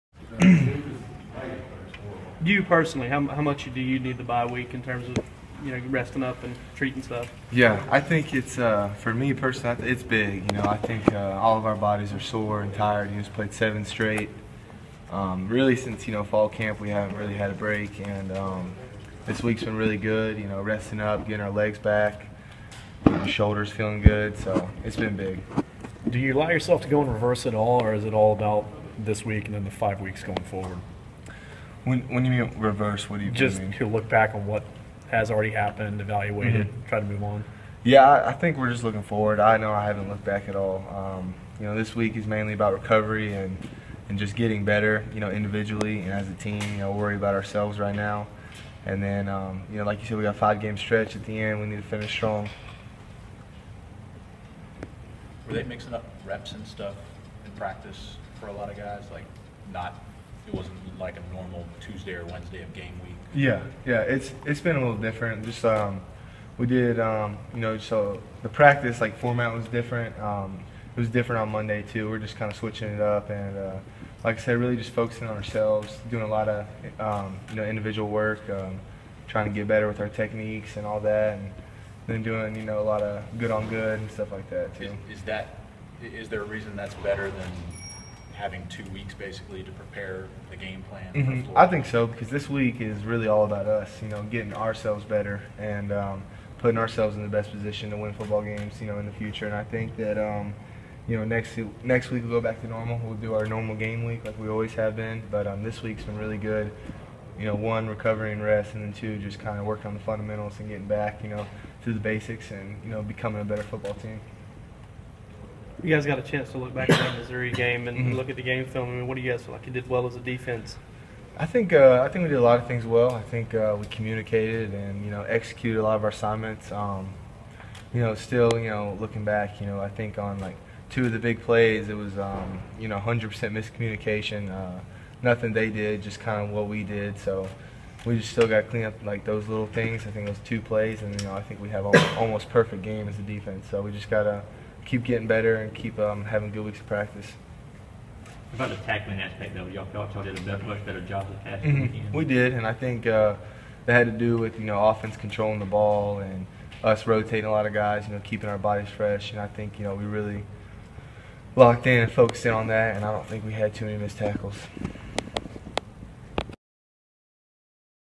Video: Jake Ganus talks bye week practice
Georgia senior inside linebacker Jake Ganus met with the media during the bye week and addressed Georgia's altered practice philosophy during the off week, which focused more on fundamental work and ones-versus-ones competitive drills.